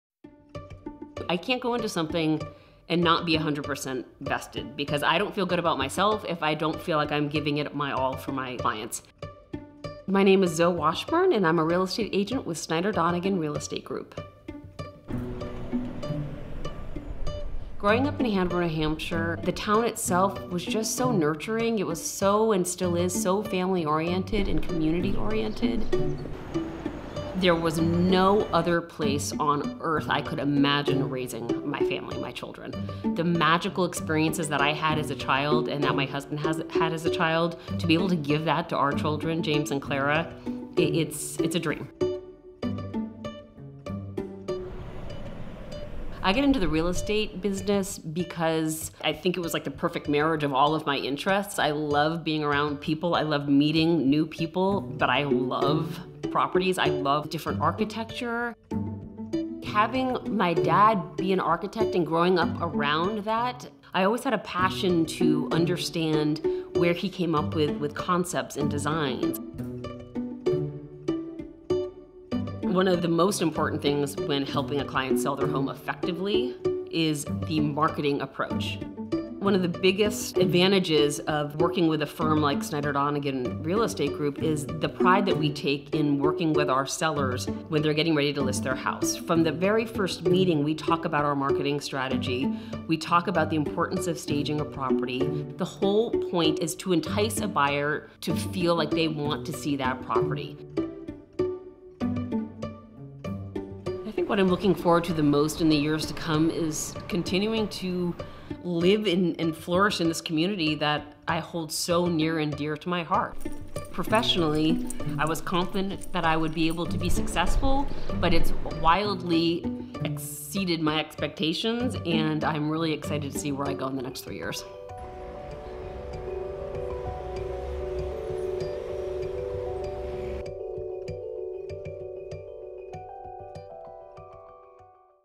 Team SD: This is Zoe
Get to know what makes Zoe Hathorn Washburn such a valued member of the Snyder Donegan Team.